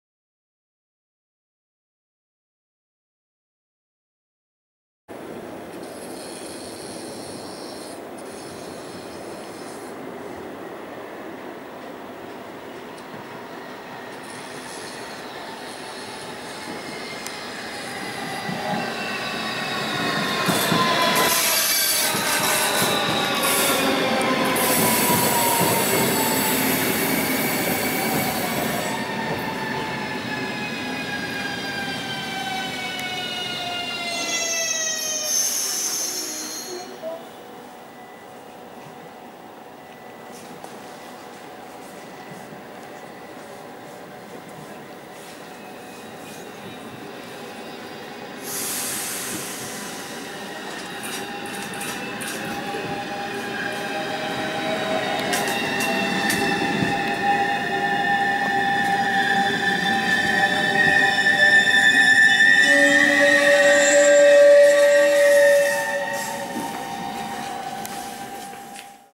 Электропоезда ЭС2Г-019 и ЭС2Г-032 Ласточка станция "МЦК" Андроновка
Пассажирский электропоезд постоянного тока ЭС2Г-019 "Ласточка" (приписан ТЧ-96 Подмосковное) с рейсом по часовой стрелке, следует по 1 пути.
Пассажирский электропоезд постоянного тока ЭС2Г-032 "Ласточка" (приписан ТЧ-96 Подмосковное) с рейсом против часовой стрелке, следует по 2 пути.
Россия, г. Москва, Московская ж/д, Московское Центральное Кольцо, станция (пассажирская) Андроновка. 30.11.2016 г.